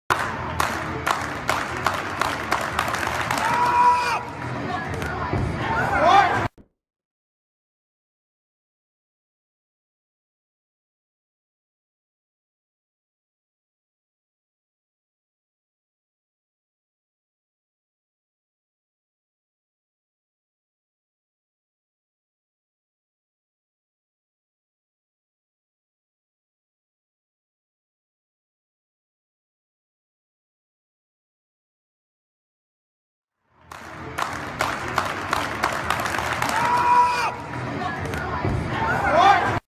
Shot Put - Jordan Giest slo motion
JORDAN GIEST who threw over 76 feet in high school and now as a University of Arizona freshman has thrown 70'3" in his first meet with the 16lb shot. Great technique slo motion video.